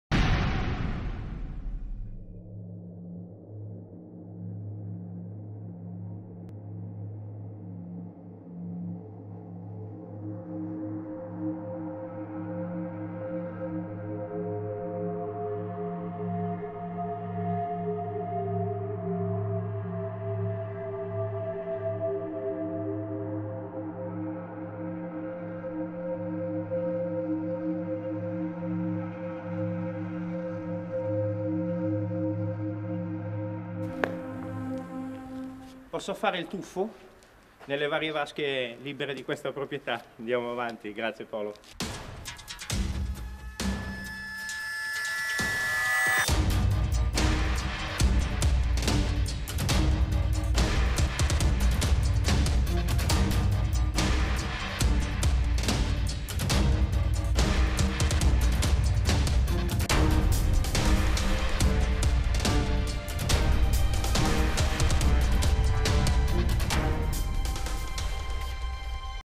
Promo ++ECO-MOSTRO CON SORPRESA ++
Dopo numerose segnalazioni , andiamo a vedere che succede alla ex Garzanti -Verga ,  chiusa da oltre un decennio e sita in Via Mazzini a Cernusco sul Naviglio .
Una maxi esplorazione da 24000 mq , con molte sorprese...